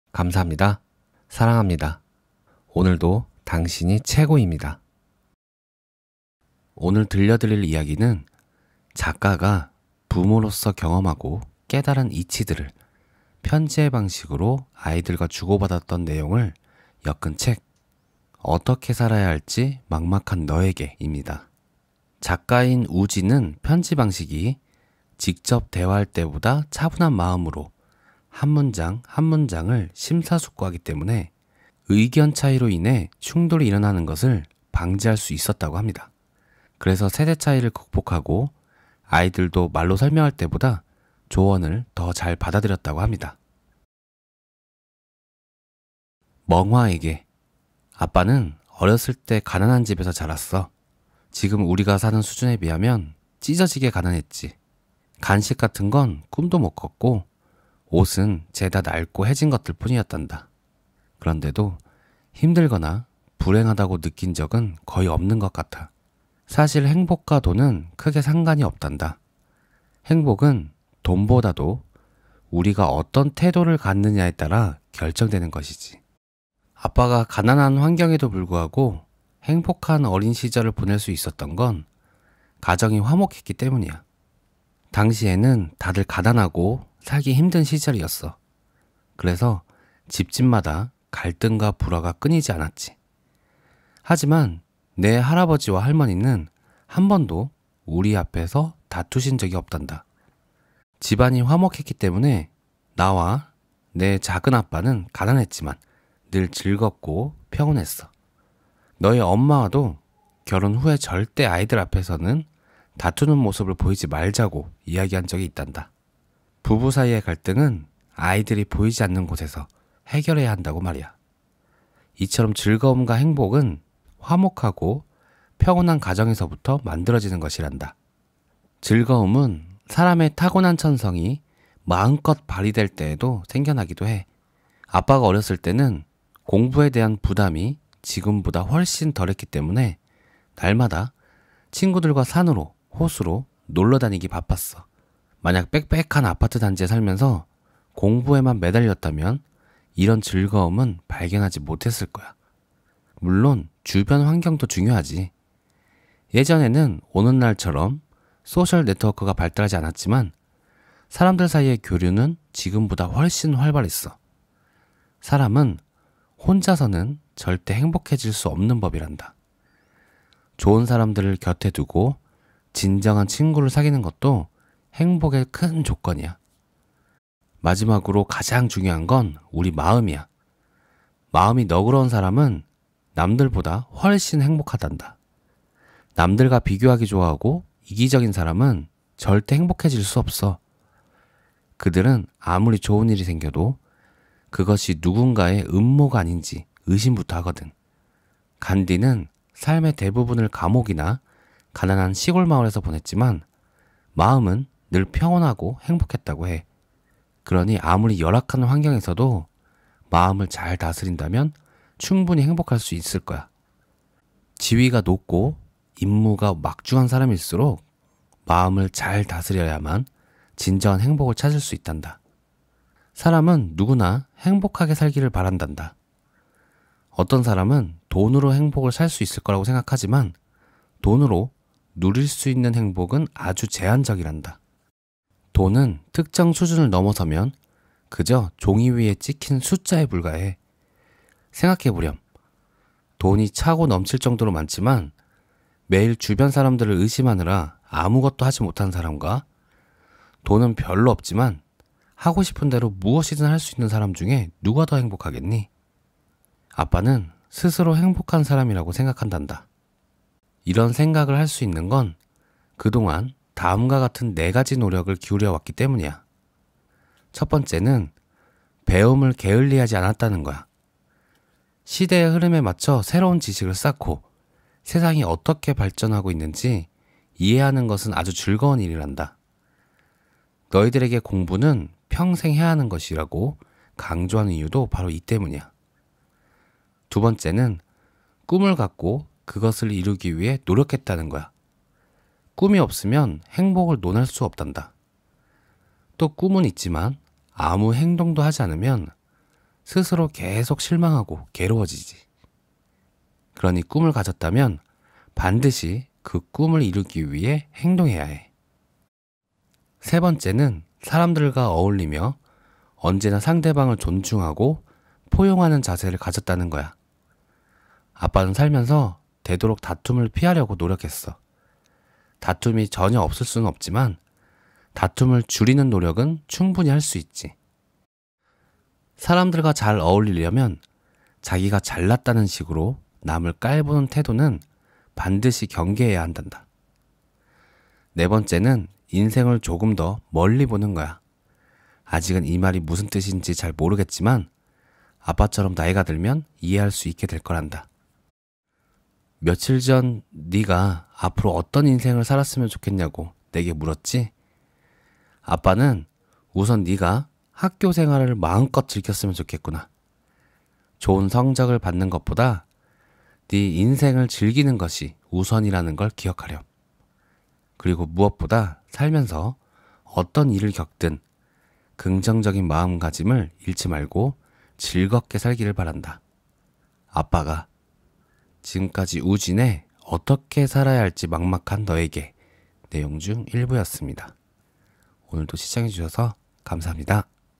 긍정적인 태도가 중요하다는 것을 세대 차이를 극복하고 알려주는 방법 | 어떻게 살아야 할지 막막한 너에게, 우쥔
아무리 열악한 환경에서도 마음을 잘 다스린다면 충분히 행복할 수 있을 거야. 지위가 높고 임무가 막중한 사람일수록 마음을 잘 다스려야만 진정한 행복을 찾을 수 있단다.

* 댓글과 좋아요 감사합니다.
* 써니즈 구독자님 사랑합니다.

참고서적
어떻게 살아야 할지 막막한 너에게 - 우쥔